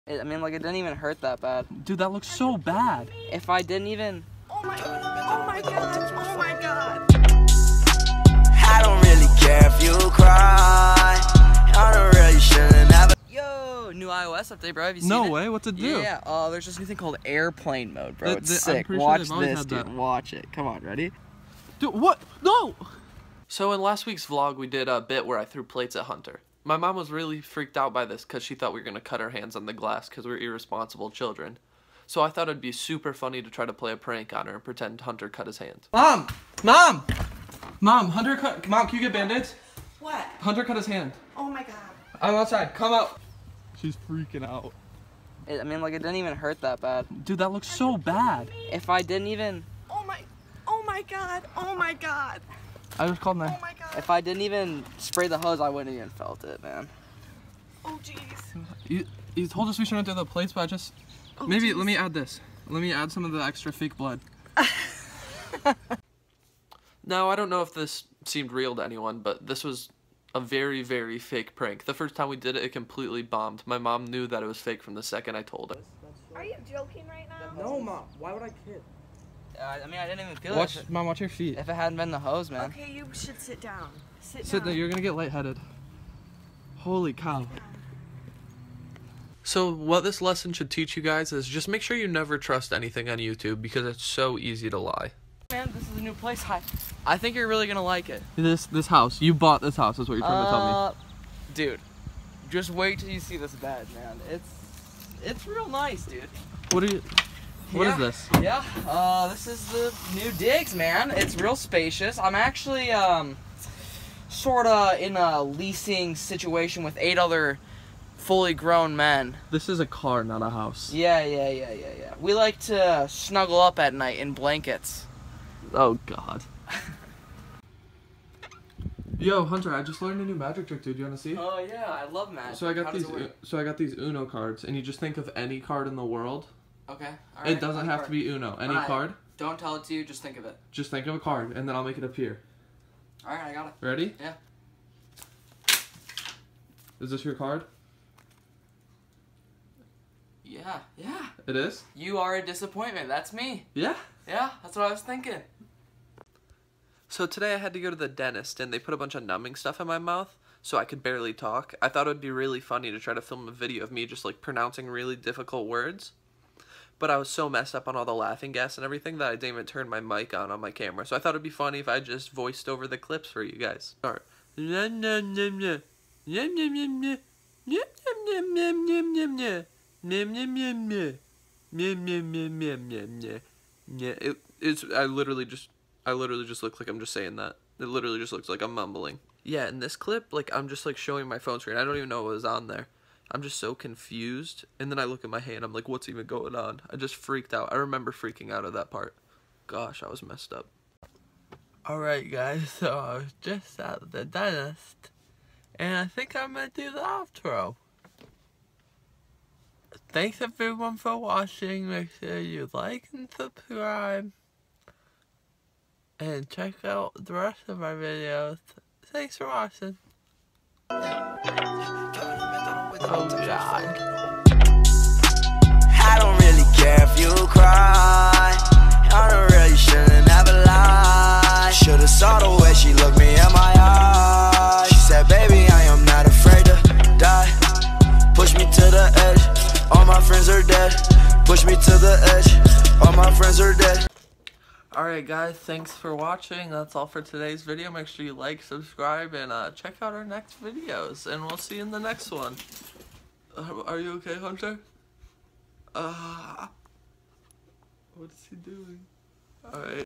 HE CUT HIS HAND SO BAD!?!?
hunter learns about airplane mode, we pull a fake blood prank on my mom, hunter shows off his new crib, payton does a magic trick, and payton goes to the dentist!

USE CODE "HunterPayton" on gamebright.co !